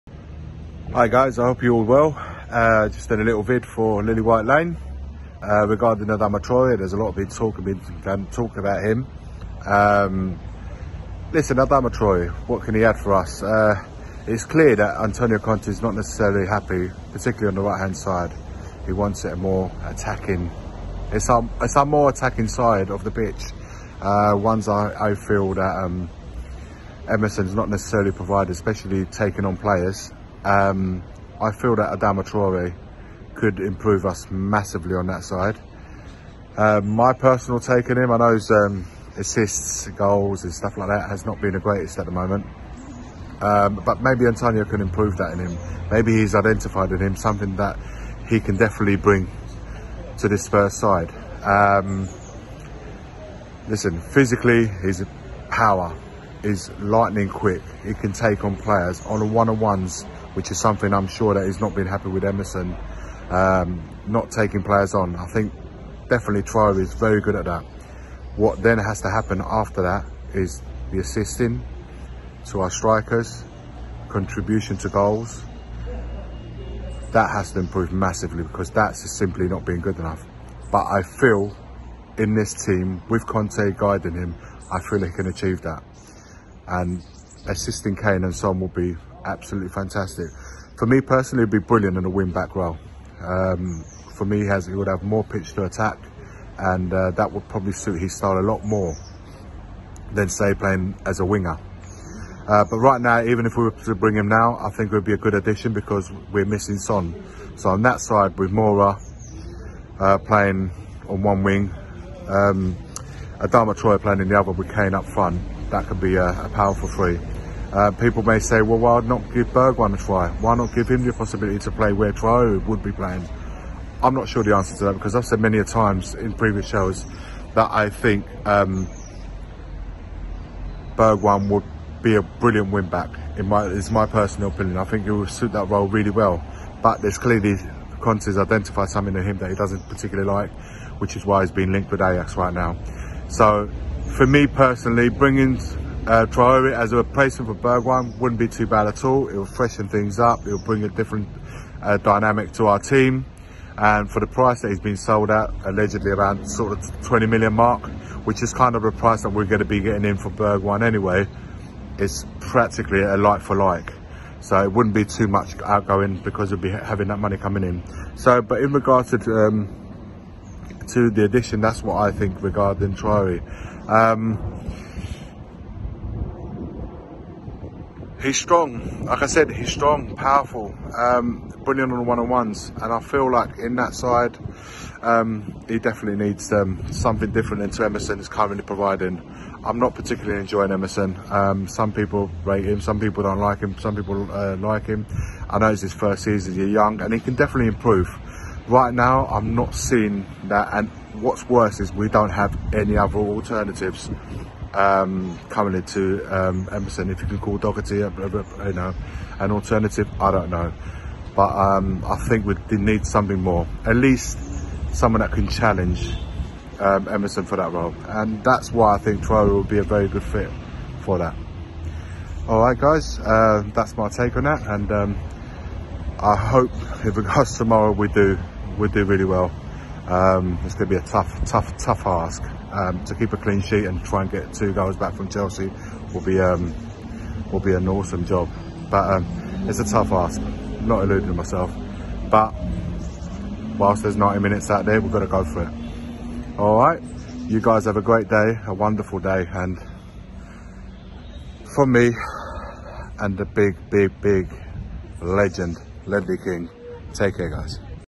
IS TRAORE A GOOD FIT FOR SPURS
Watch as igy from @TottenhamAway  brings to you a video discussing and giving his opinion on the potential transfer for Adama Traore to come to Tottenham👍